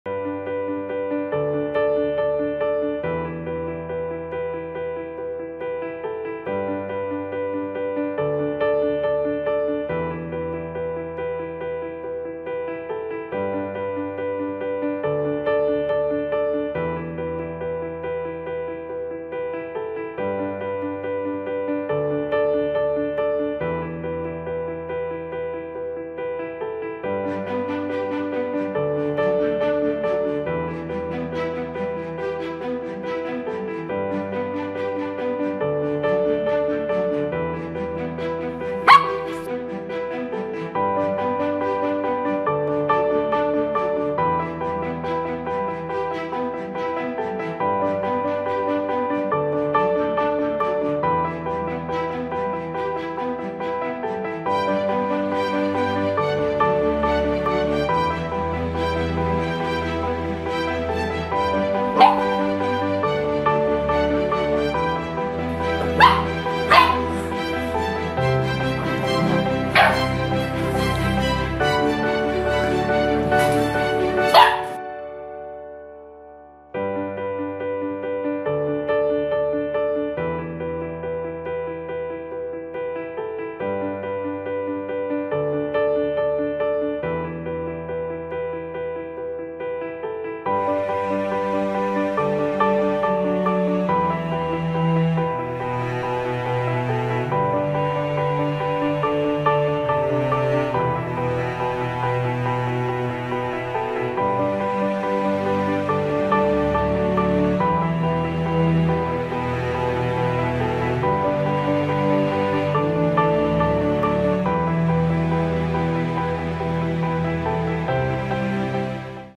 Goldendoodle Maya ist nun eine Woche bei uns
Nach dem Tod unseres Labradoodle Maggie haben wir uns für einen neuen Hund entschieden. Und das ist Maya